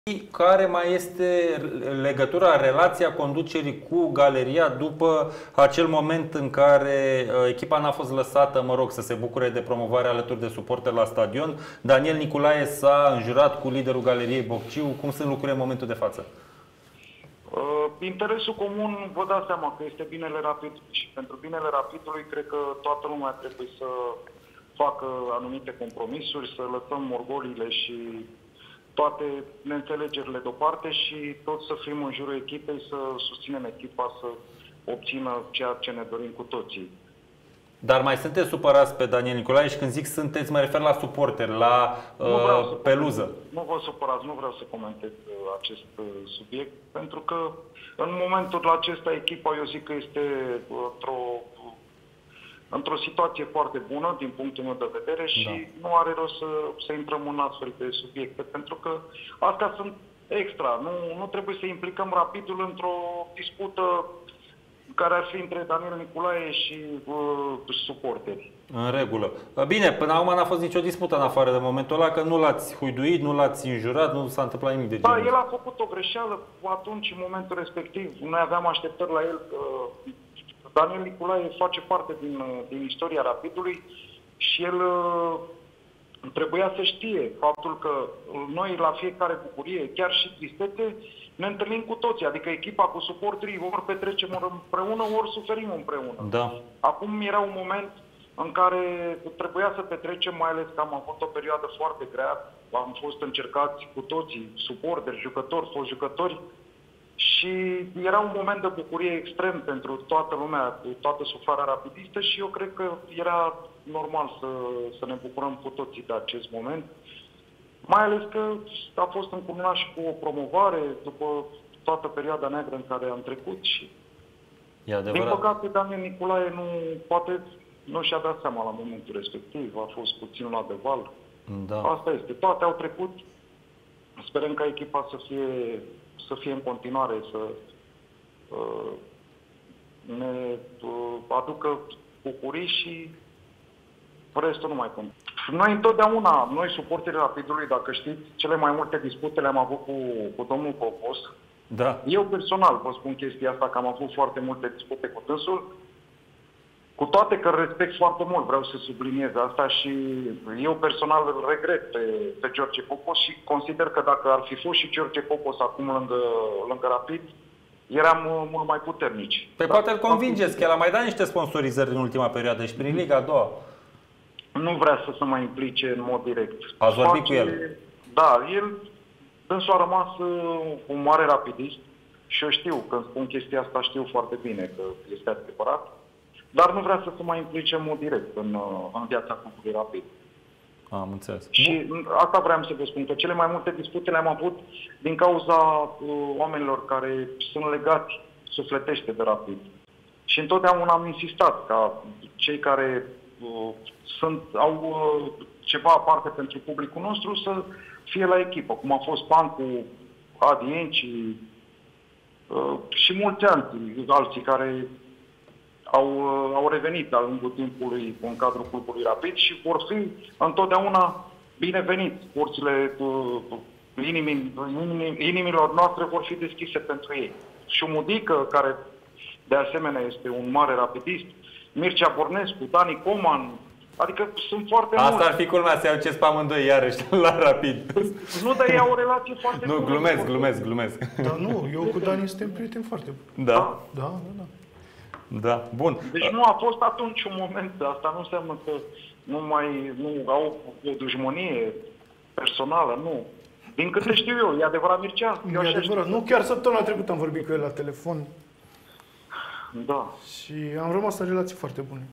Revine George Copos la Rapid ca patron al clubului giuleștean? Anunțul lui Gigi Corsicanu
Gigi Corsicanu, celebrul lider de galerie al celor de la Rapid București, a dezvăluit care sunt șansele ca George Copos să revină ca patron în Giulești în perioada următoare.